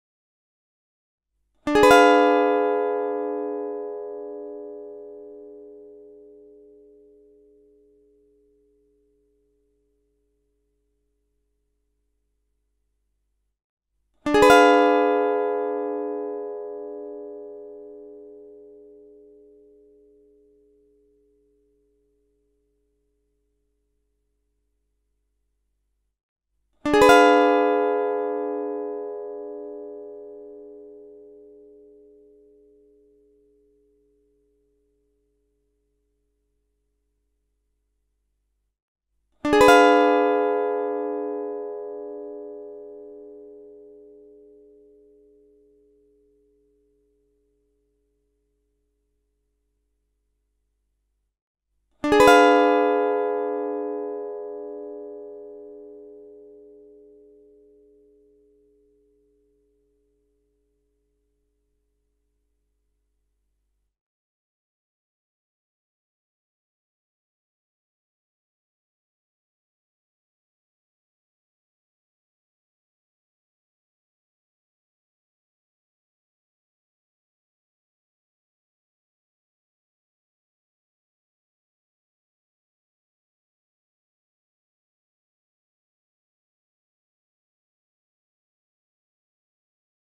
Guitar Chord: Ddim7 (vi) (x x 12 13 12 13)
Third Barred "Ddim7" Guitar Chord [12th Fret] on Standard Tuning (EADGBE) played on Western Guitar for your personal Guitar Tuning!!!

TAB:
E x
A x
D 12
G 13
B 12
E 13